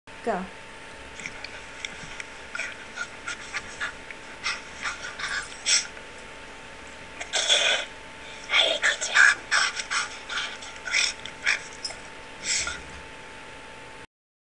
Que Laugh!
Dr. Emily Weiss discusses this form of canine communication in her Shelters' Edge blog post